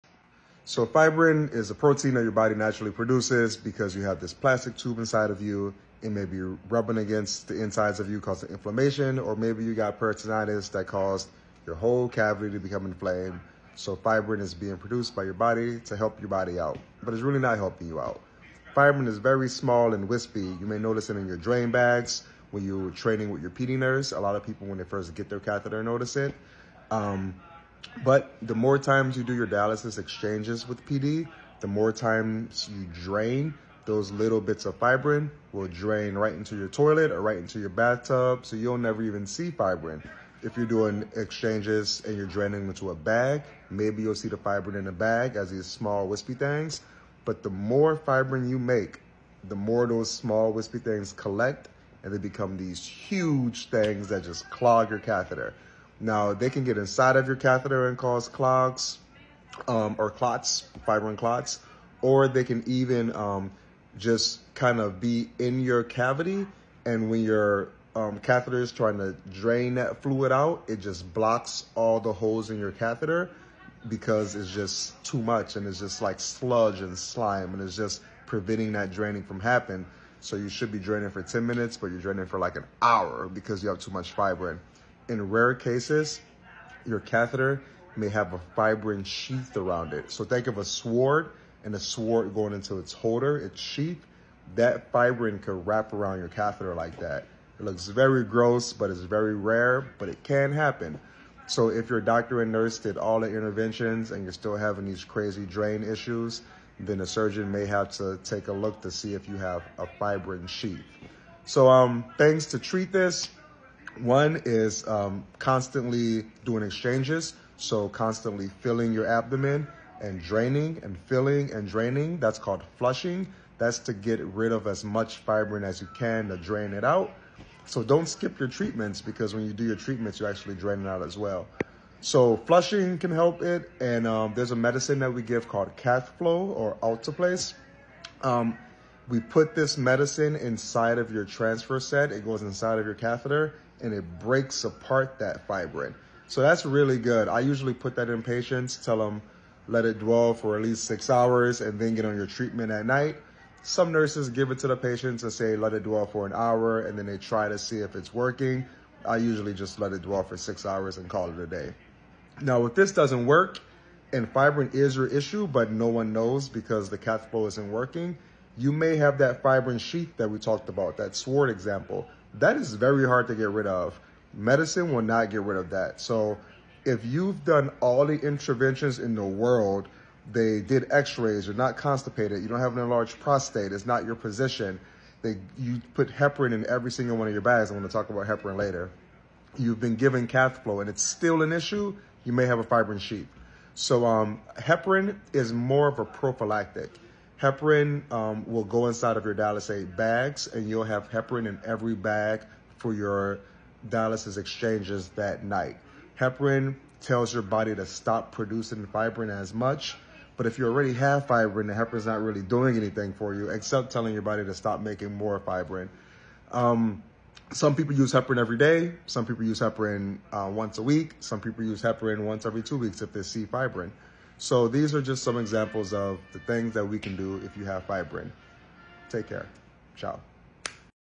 How to get rid of fibrin
Explanation of fibrin and how to reduce its formation and the medications used to disintegrate it. Also how to solve the drain issue question when you have a fibrin sheathe